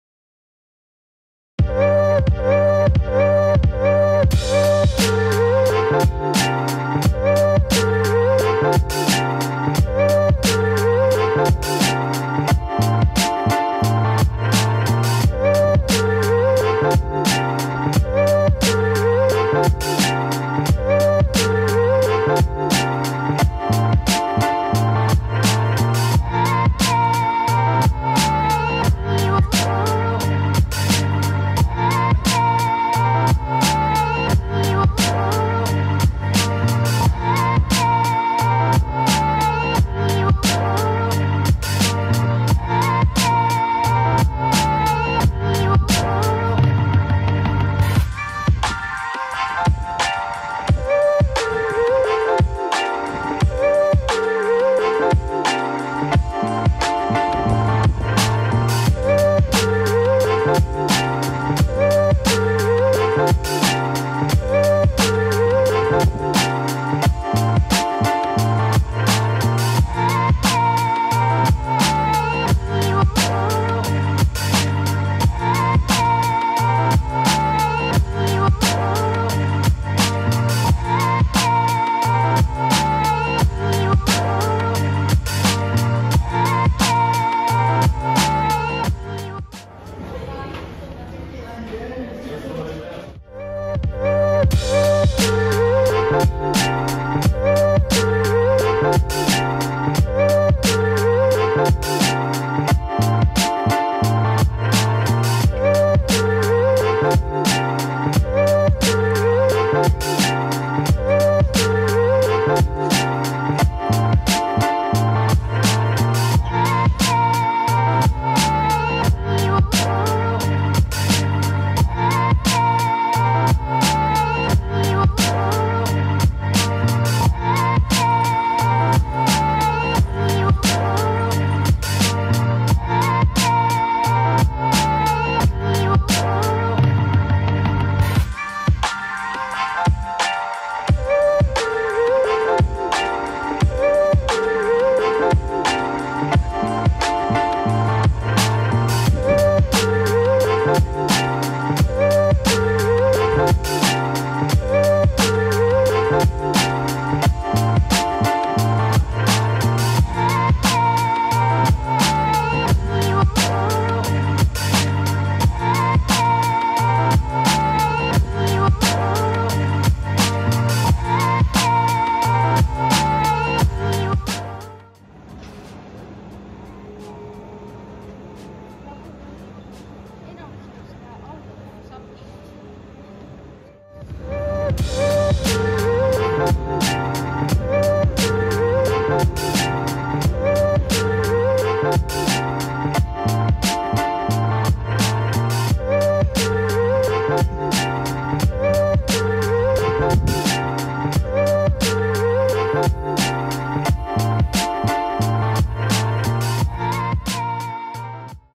Helsinki & Tallinn, Two of the Most Underrated Cities in Europe
Hello there! This is a short guide of what to visit in two of the more underrated cities in Europe. I spent three days in Helsinki and two in Tallinn which should give you a good overview of the two. Let me know if you have questions!